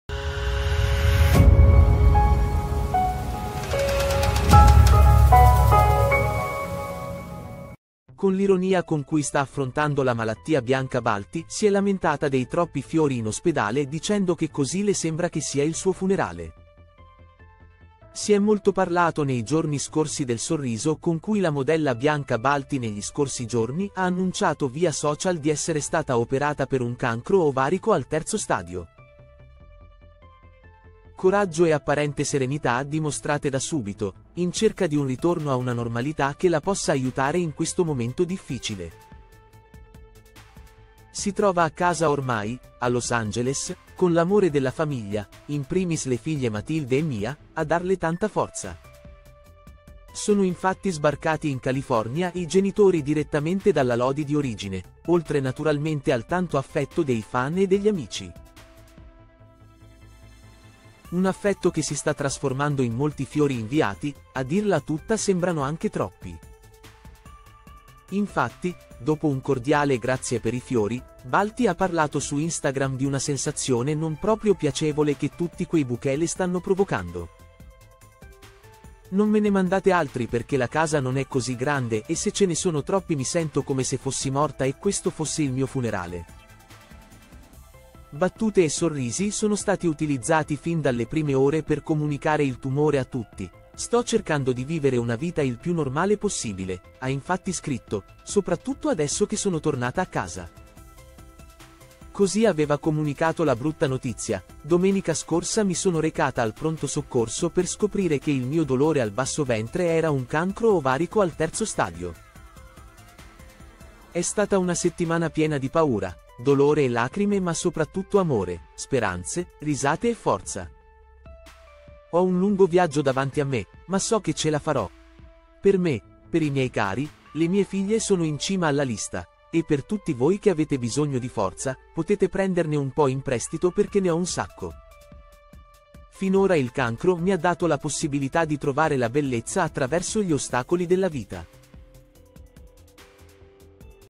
Bianca Balti non vuole troppi fiori, ma tanti sorrisi
Con l'ironia con cui sta affrontando la malattia Bianca Balti si è lamentata dei troppi fiori in ospedale dicendo che così le sembra che sia il suo funerale.
Si è molto parlato nei giorni scorsi del sorriso con cui la modella Bianca Balti negli scorsi giorni ha annunciato via social di essere stata operata per un cancro ovarico al terzo stadio. Coraggio e apparente serenità dimostrate da subito, in cerca di un ritorno a una normalità che la possa aiutare in questo momento difficile. Si trova a casa ormai, a Los Angeles, con l'amore della famiglia, in primis le figlie Matilde e Mia, a darle tanta forza. Sono infatti sbarcati in California i genitori direttamente dalla Lodi di origine, oltre naturalmente al tanto affetto dei fan e degli amici.